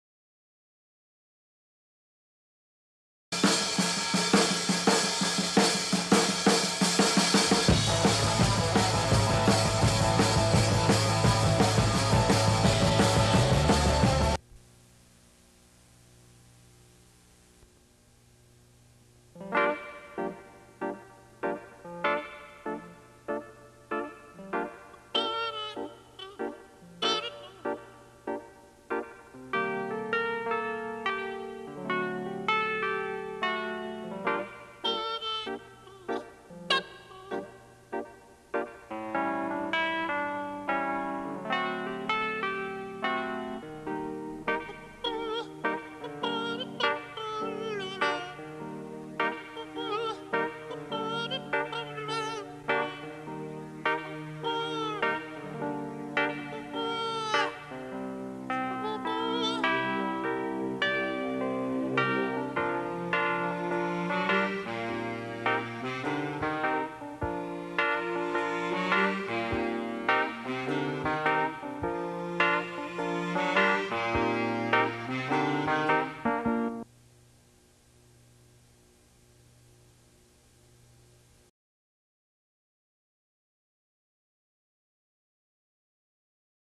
Animation Tests-1979
This was from test footage before we filmed "The Worm Family Goes to the Beach" done on Super 8mm film in 1979 while attending Brooks Photography Institute in Santa Barbara, CA. This was another collaborative effort with Greg Kopacka and shot at our apartment complex and the tennis courts right next to our complex in Santa Barbara. I'm the guy with the red hair!